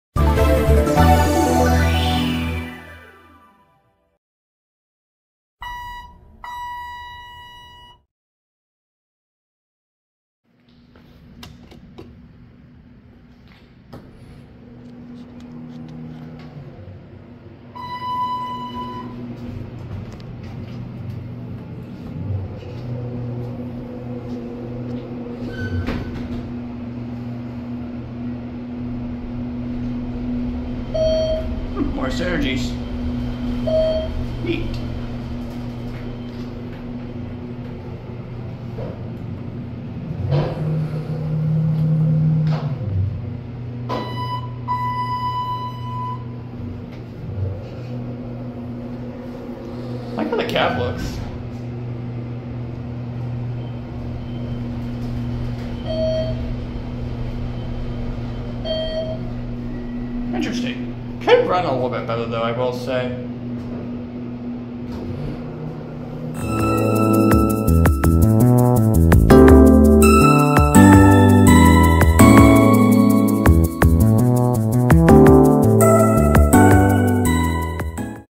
Thyssenkrupp Synergy Traction Elevators - UT Haslam Music Center (Knoxville, TN)
Here is some neat older TK Synergies with MAD. They don't run the best, but the cabs are nice.

Elevator Brand: ThyssenKrupp
Elevator Model (if identifiable): Synergy
Elevator Type: Traction
Number of Elevators: 2
Floors Served: 4
Installed: 2012
Current Controller: TAC50
Capacity: 3500 lbs
Speed: 350 FPM

Original Fixtures:
Buttons: MAD BS Classic
Floor Indicator: TK Digital Red Dot Matrix
Directional Lanterns: TK Vandal Resistant
Chimes/Bells: Dover Electronic / TKE Grocery Beep

Recorded: May 24th, 2024
Phone Used: Samsung Galaxy S23 Ultra
Chime Sampled by yours truly and edited on Audacity
Video Edited on Clipchamp